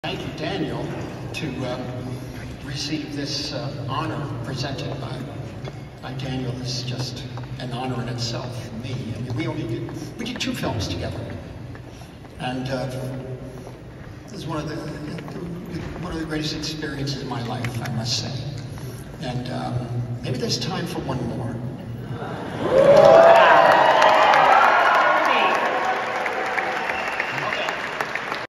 Martin Scorsese teases another film with Daniel Day-Lewis
Martin Scorsese surprised the crowd at the National Board of Review Awards when he suggested he and Daniel Day-Lewis work together one more time. This week the actor took a break from retirement to present Scorsese with the award for best director at the gala.